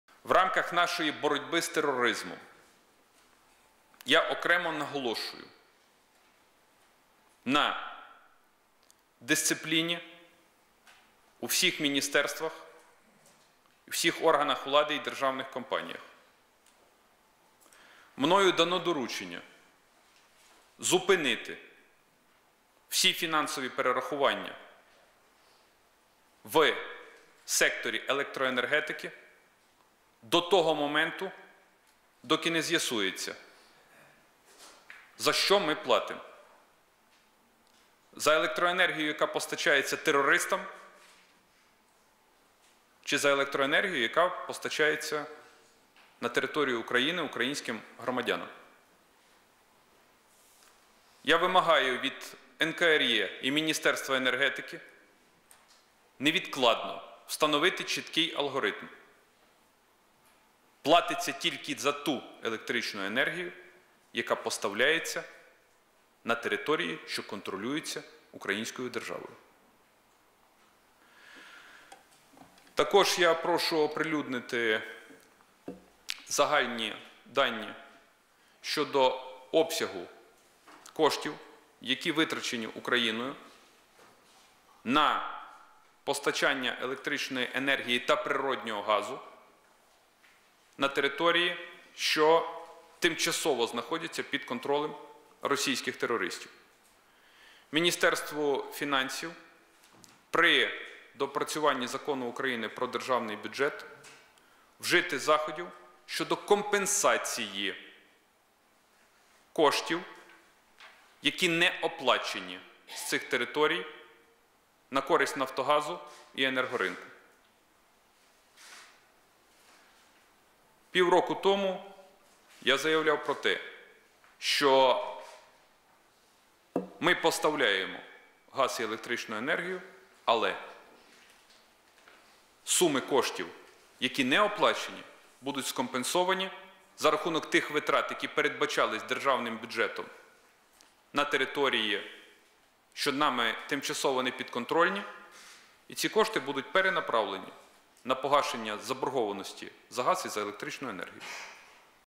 Яценюк: Сплачувати лише за електроенергію, яка постачається на території підконтрольні Україні
Прем’єр-міністр України, лідер партії «Народний фронт» Арсеній Яценюк вимагає від НКРЕ і Міністерства енергетики невідкладно встановити чіткий алгоритм – «платиться тільки за ту електричну енергію, яка поставляється на території, що контролюються українською державою».
Про це Глава Уряду заявив на засіданні Кабінету Міністрів у середу, 18 січня.

Арсеній Яценюк дав доручення зупинити фінансові перерахування, «доки не з'ясується, за що ми платимо - за електроенергію, яка постачається терористам,чи яка постачається на територію України українським громадянам».

Глава Уряду також доручив оприлюднити загальні дані щодо обсягу коштів, витрачених Україною на постачання електричної енергії та природного газу на території, що тимчасово знаходяться під контролем російських терористів.

Міністерству фінансів він доручив при доопрацюванні закону про Державний бюджет вжити заходів щодо компенсації коштів, які не оплачені з цих територій, на користь «Нафтогазу» і енергоринку: «Півроку тому я заявляв, що ми поставляємо газ і електричну енергію, але суми коштів, які не оплачені, будуть скомпенсовані за рахунок витрат, які передбачалися Державним бюджетом на території, що нам тимчасово непідконтрольні. І ці кошти будуть перенаправлені на погашення заборгованості за газ і електричну енергію».

«У рамках боротьби з тероризмом я окремо наголошую на дисципліні у всіх міністерствах, у всіх органах влади і державних компаніях», - підкреслив Прем’єр-міністр.

Приєднуйся!